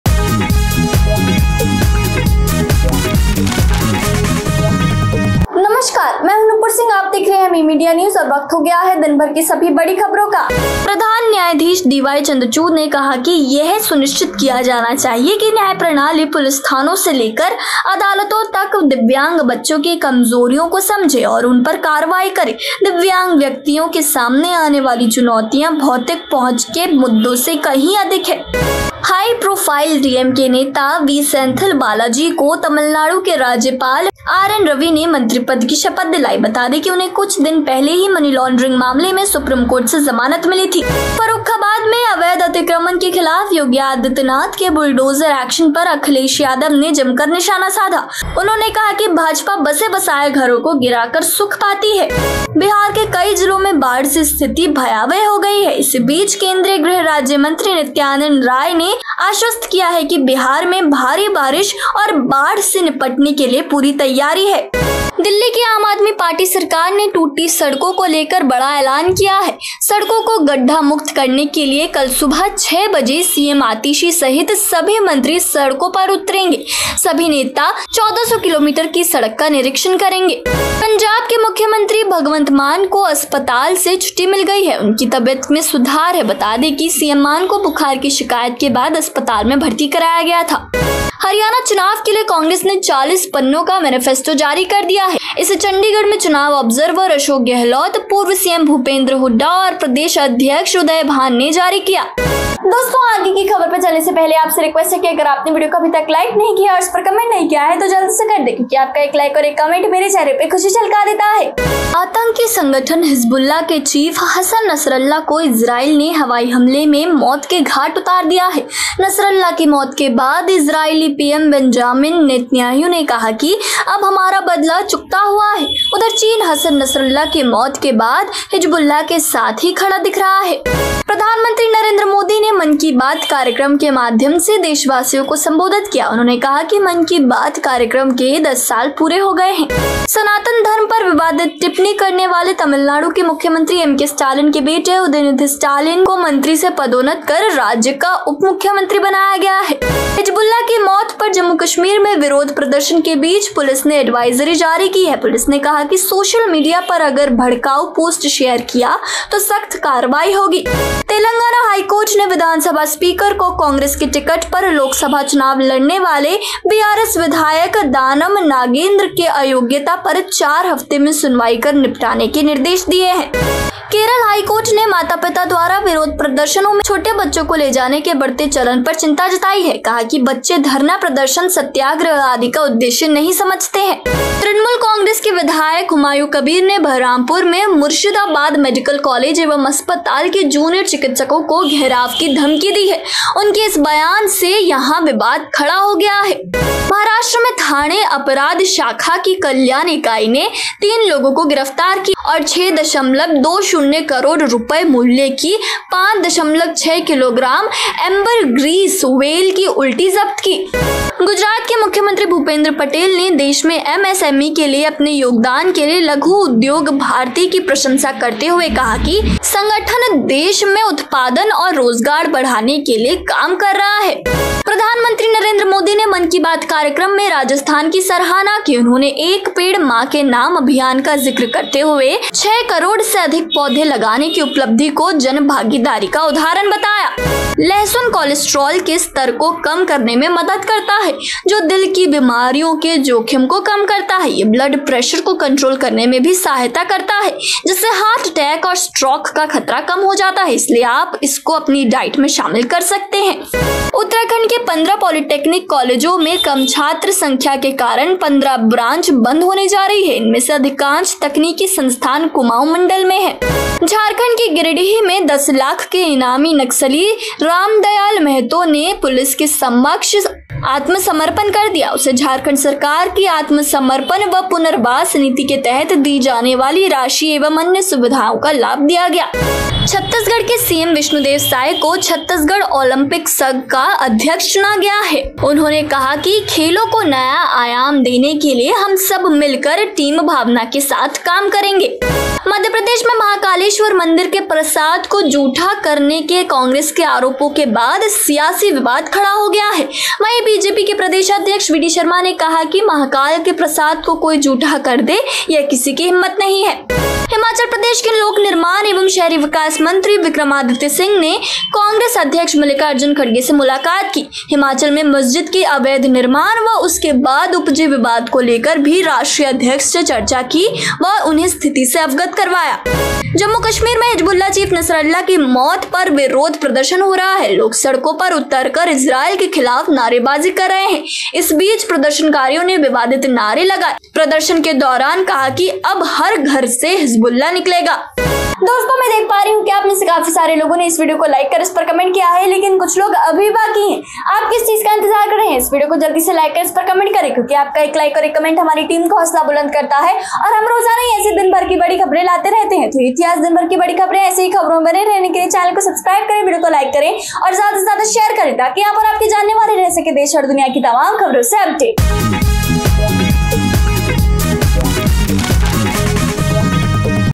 29 September Evening News Bulletin | Desh Duniya | Nonstop News | aaj ka Samachar | Mi Media News 24
29 September Evening News Bulletin | Desh Duniya | Nonstop News | aaj ka Samachar | Mi Media News 24

 आतंकी संगठन हिजबुल्लाह के चीफ हसन नसरल्लाह को इजरायल ने हवाई हमले में मौत के घाट उतार दिया है।

प्रधानमंत्री नरेंद्र मोदी मन की बात कार्यक्रम के माध्यम से देशवासियों को संबोधित किया।

सनातन धर्म पर विवादित टिप्पणी करने वाले तमिलनाडु के मुख्यमंत्री एमके स्टालिन के बेटे उदयनिधि स्टालिन को मंत्री से पदोन्नत कर राज्य का उपमुख्यमंत्री बनाया गया है।

हिजबुल्लाह की मौत पर जम्मू-कश्मीर में विरोध प्रदर्शन के बीच पुलिस ने एडवाइजरी जारी की है।

तेलंगाना हाई कोर्ट ने विधानसभा स्पीकर को कांग्रेस के टिकट पर लोकसभा चुनाव लड़ने वाले बीआरएस विधायक दानम नागेंद्र के अयोग्यता पर चार हफ्ते में सुनवाई कर निपटाने का निर्देश दिए है।

केरल हाई कोर्ट ने माता-पिता द्वारा विरोध प्रदर्शनों में छोटे बच्चों को ले जाने के बढ़ते चलन पर चिंता जताई है।

प्रधान न्यायाधीश डीवाई चंद्रचूड़ ने कहा कि यह सुनिश्चित किया जाना चाहिए कि न्याय प्रणाली पुलिस थानों से लेकर अदालतों तक दिव्यांग बच्चों की कमजोरियों को समझे और उन पर कार्रवाई करे।

Hassan Nasrallah, the chief of the terrorist organization Hezbollah, has been killed in an airstrike by Israel.

Prime Minister Narendra Modi addressed the countrymen through the Mann Ki Baat program.

Udhayanidhi Stalin, son of Tamil Nadu Chief Minister MK Stalin, who made controversial remarks on Sanatan Dharma, has been promoted from minister to Deputy Chief Minister of the state.

Amid protests in Jammu and Kashmir over the death of Hezbollah, the police have issued an advisory.

The Telangana High Court has directed the Assembly Speaker to hear and settle the disqualification of BRS MLA Danam Nagendra, who contested the Lok Sabha elections on a Congress ticket, within four weeks.

The Kerala High Court has expressed concern over the increasing trend of parents taking young children to protests.

Chief Justice DY Chandrachud said that it should be ensured that the justice system, from police stations to courts, understands the vulnerabilities of disabled children and takes action on them.